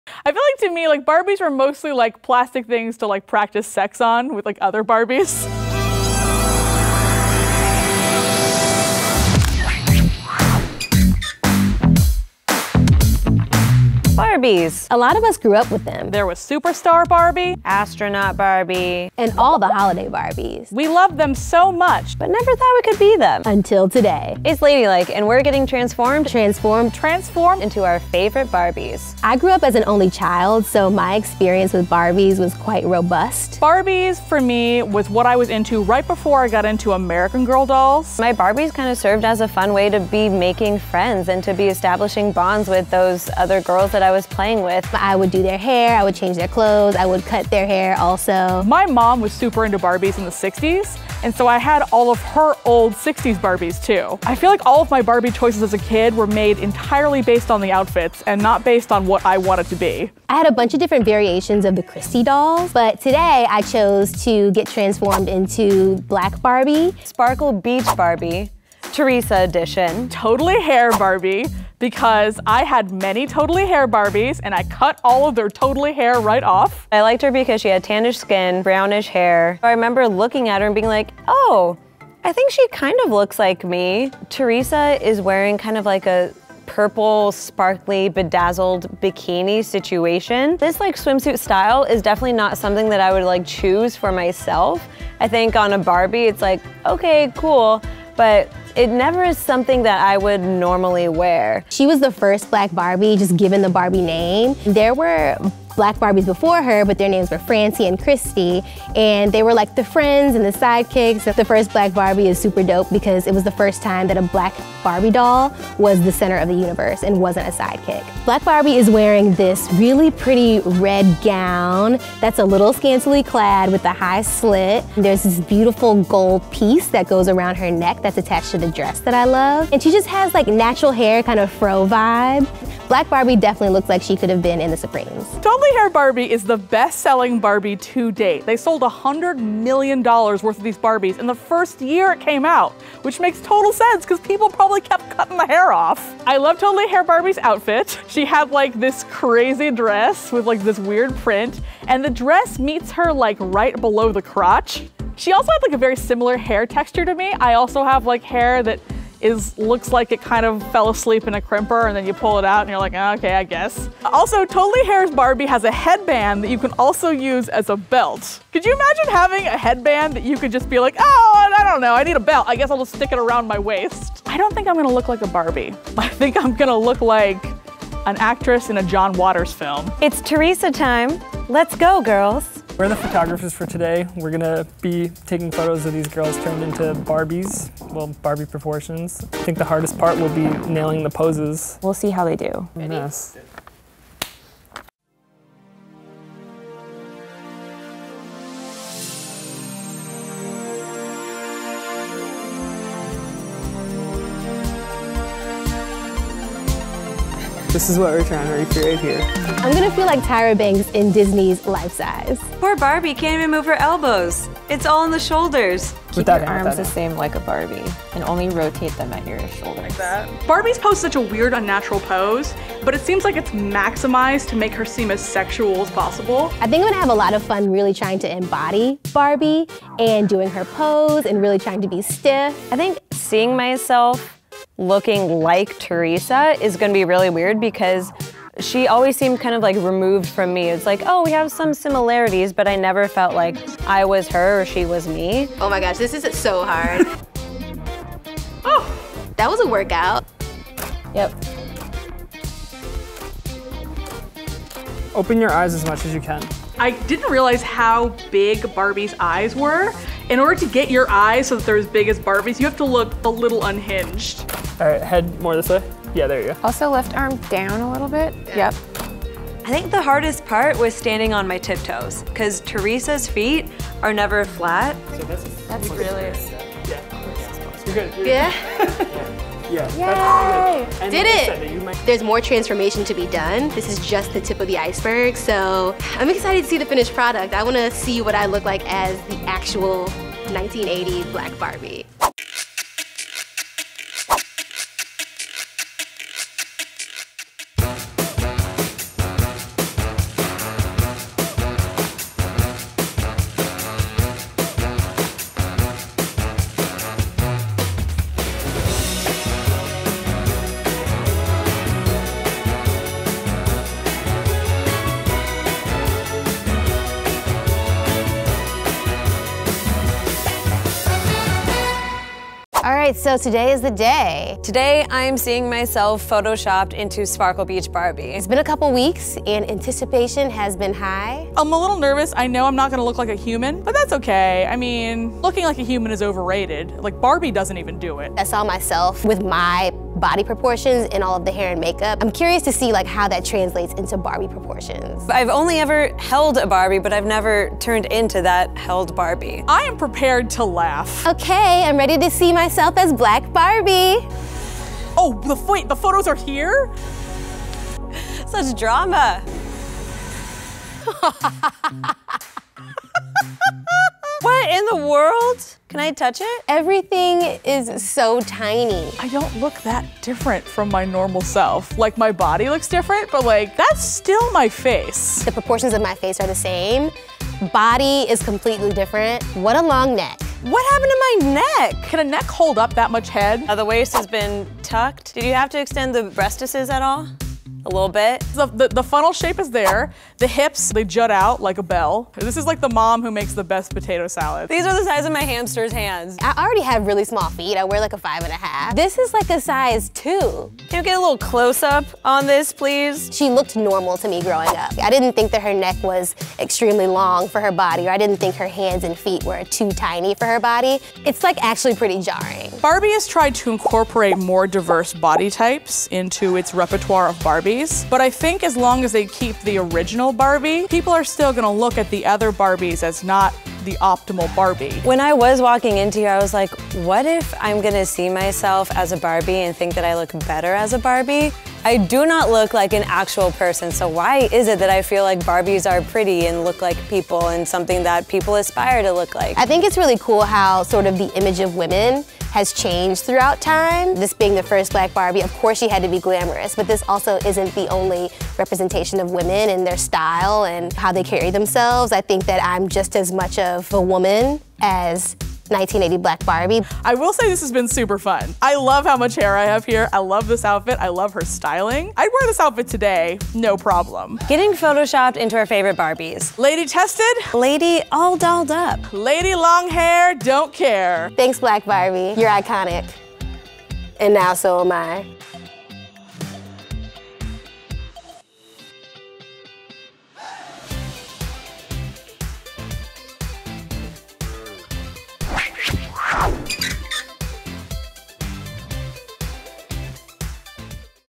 We Got Photoshopped Into Our Favorite Barbies • Ladylike
"I'm gonna feel like Tyra Banks in Disney's Lifesize"

Ladylike
We are so excited to have you here. Lady test, lady stay-tuned.

External Credits:
Costume Designer: Nadia Beeman